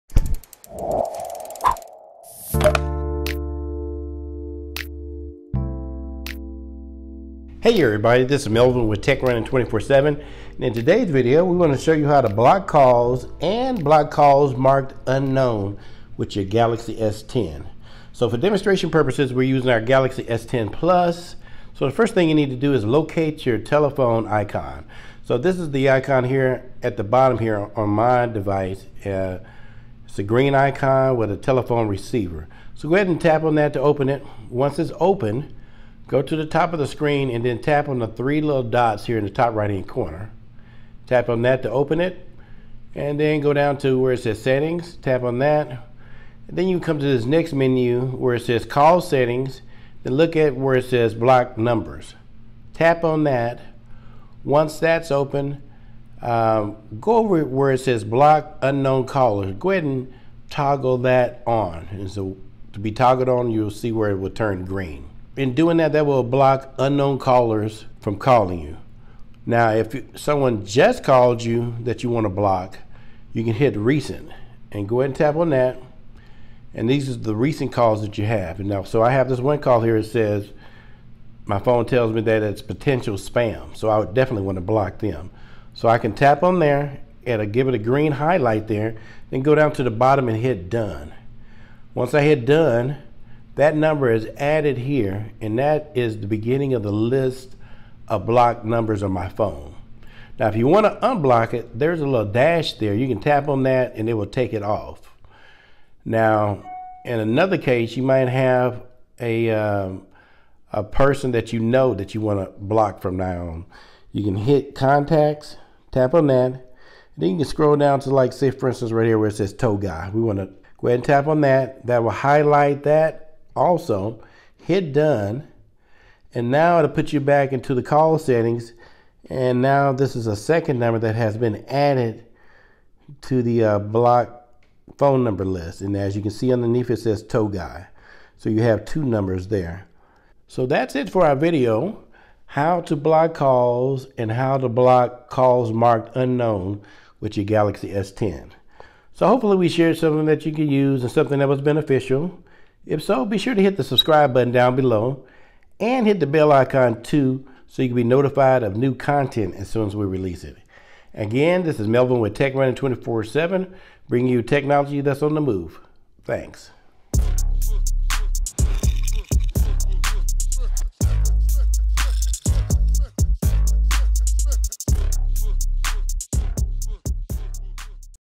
Galaxy S10/S10 Plus: How To Block Calls and Block Calls marked Unknown!
In this video we will show you how to block call with your new device.  And we will show how to block call marked " Unknown " too.

Galaxy S10 Plus

Galaxy S10

Galaxy S10e